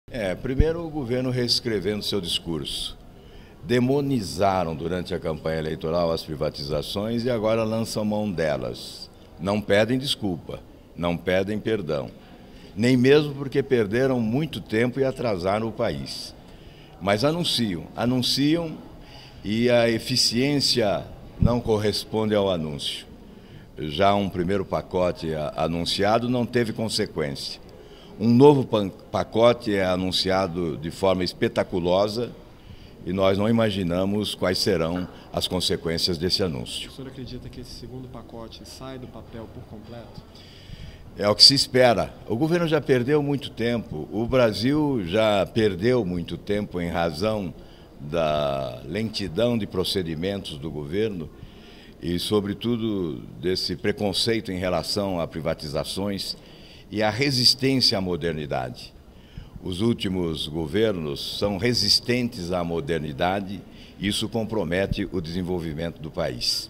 Mais um anúncio espetaculoso do Governo Federal
Em entrevista no Senado, o Líder do PSDB, senador Alvaro Dias, falou sobre o pacote de concessões de rodovias e ferrovias anunciado hoje pela presidente Dilma. "Primeiro demonizaram as privatizações, e agora lançam mão dela. É mais um anúncio feito de forma espetaculosa, lembrando que as metas do primeiro programa ainda nem foram cumpridas. O governo já perdeu muito tempo. Esperamos que dessa vez o programa seja bem sucedido"